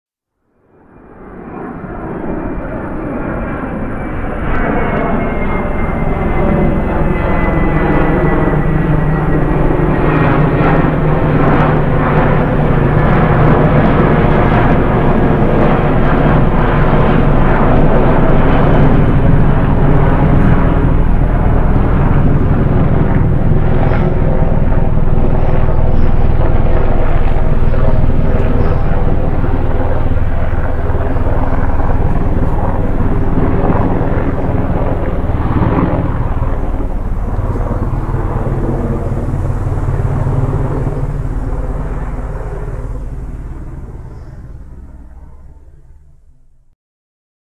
RUSSIA UKRAINE WAR UPDATE
Russia is Planning to Destroy Ukraine in August 2023